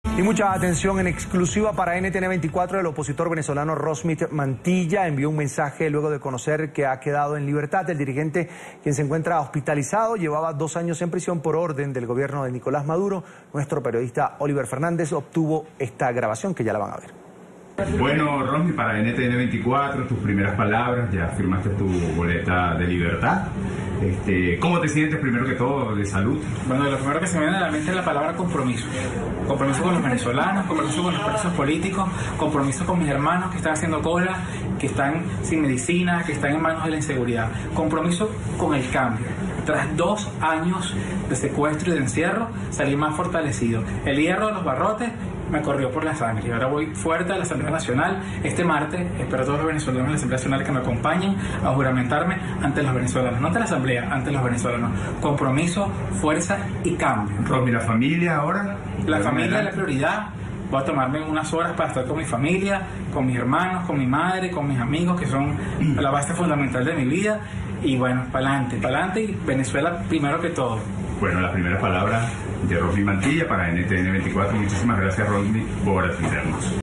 En libertad el diputado de Voluntad Popular, Rosmit Mantilla
La noche de este jueves se pudo conocer que el preso político y diputado suplente Rosmit Mantilla será liberado en las próximas horas. Su madre, Ingrid de Díaz, confirmó a NTN24 Venezuela que ya firmó la boleta de excarcelación y que se espera que mañana le den de alta en el Hospital.